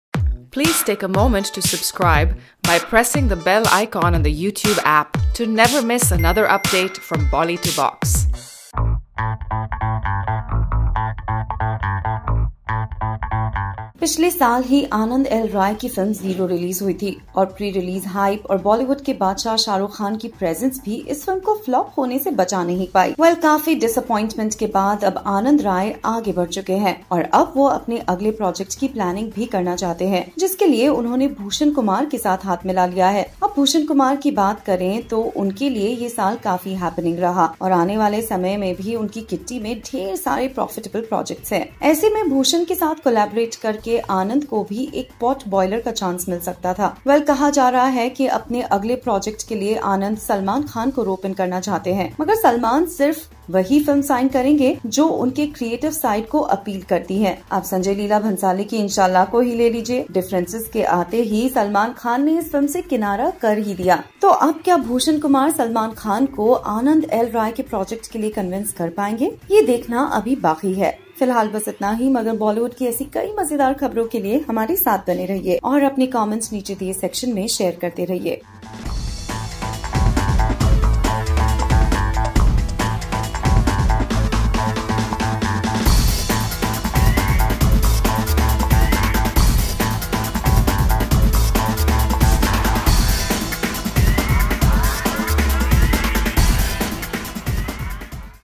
Dabangg 3 और Radhe मूवी के बाद Salman Khan डायरेक्टर Aanand L. Rai के साथ करेंगे अगली मूवी में काम
AanandLRai के साथ करेंगे अगली मूवी में काम